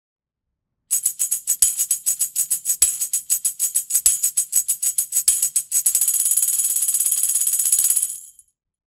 Bossa Nova Percussion - One-Row Deluxe Tambourine (8")
These One Row Deluxe Tambourines are lightweight and durable, and feature steel jingles and a rubber grip.  Available in 8" and 10" sizes.

Microphone Used: Blue Spark
Recorded at Rhythm Traders Drum Shop in Portland, OR
Video by Wesley X. Yurovchak
Audio by Ben Engen